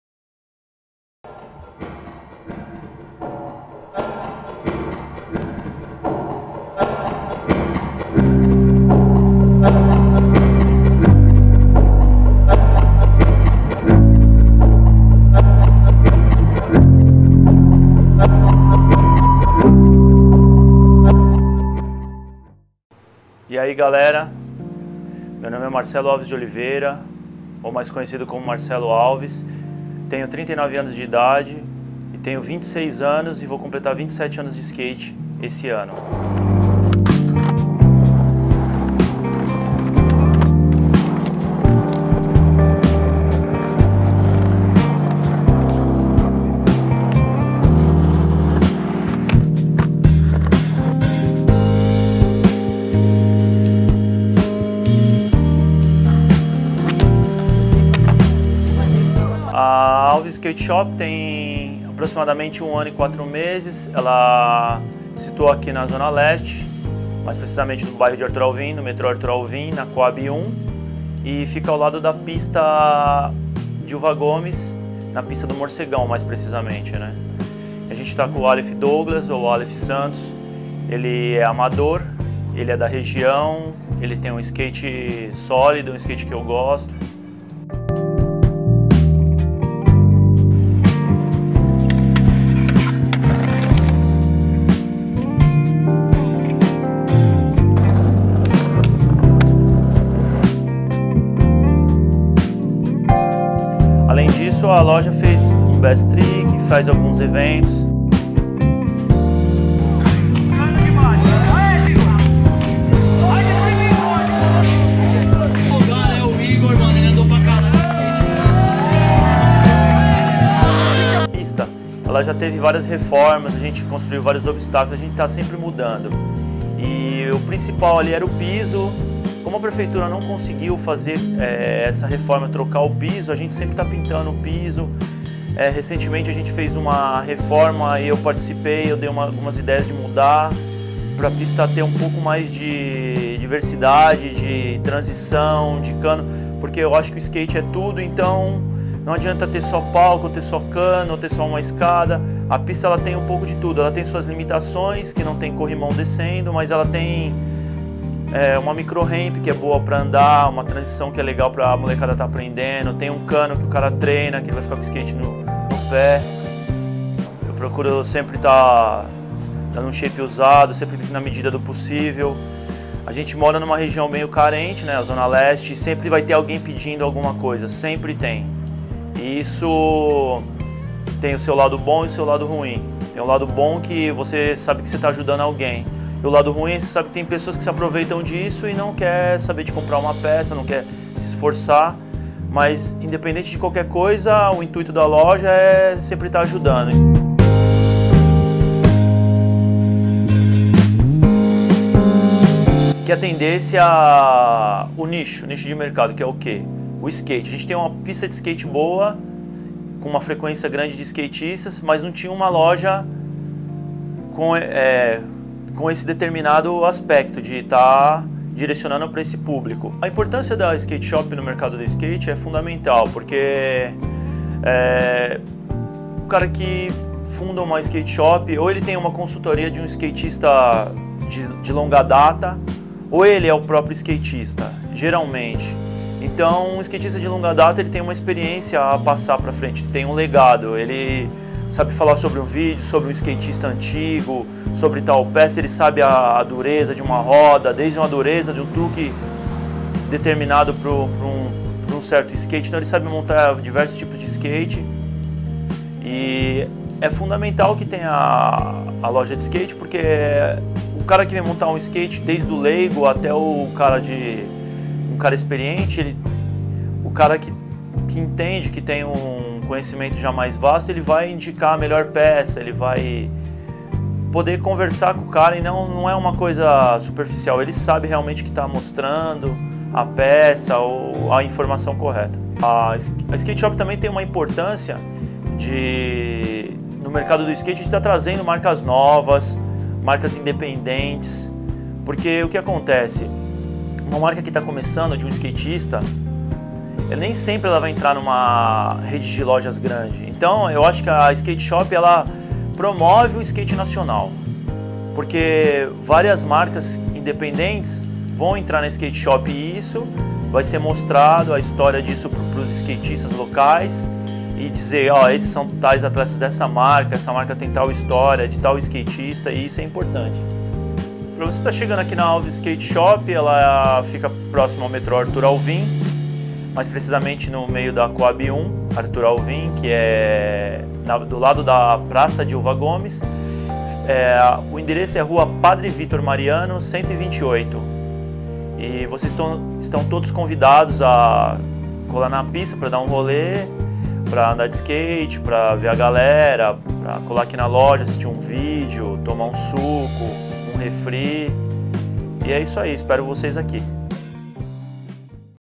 55 VM | ALVS Skateshop
55 VM  visando fortalecer as bases do skateboard nacional  apresenta a ALVS Skateshop,uma loja de skatista para skatista.